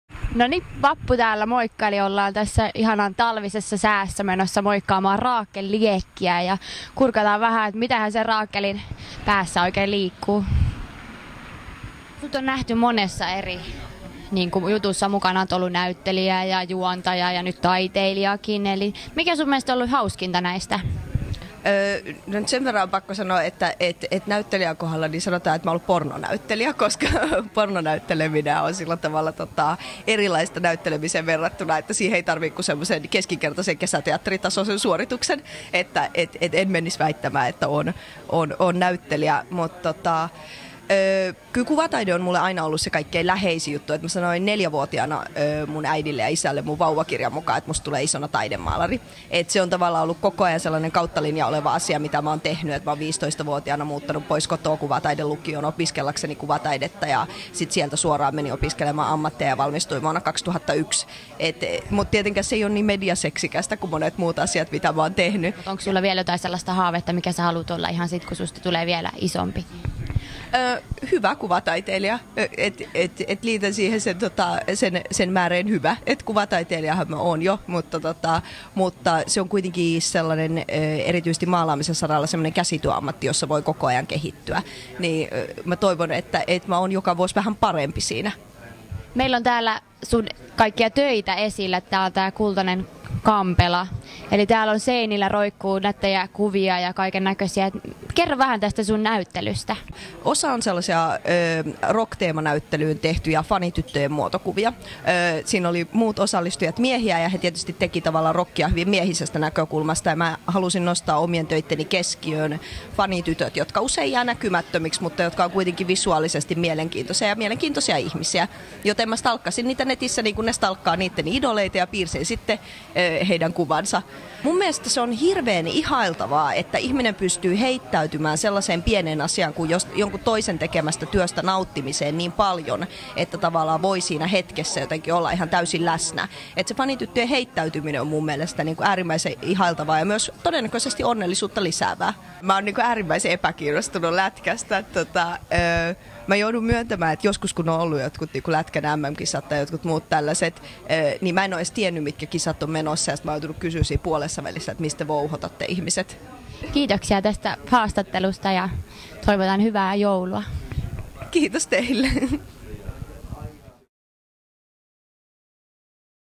Rakel Liekki haastattelu
Kuvataiteilija ja entinen pornotähti Rakel Liekki haastattelussa joulukuussa 2010 Lauttasaaren Kultaisessa Kampelassa pidetyn näyttelynsä johdosta. Haastattelijana tutkiva työpaikkajournalisti Vappu Voite Oy:stä.